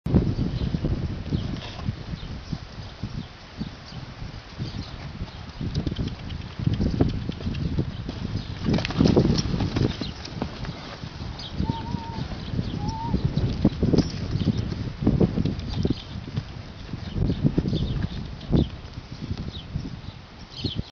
Doma primera montada el Vazco
Lo sacamos del palenque, dio media vuelta y se quiso sacar al jinete de ensima. Lo aguantamos.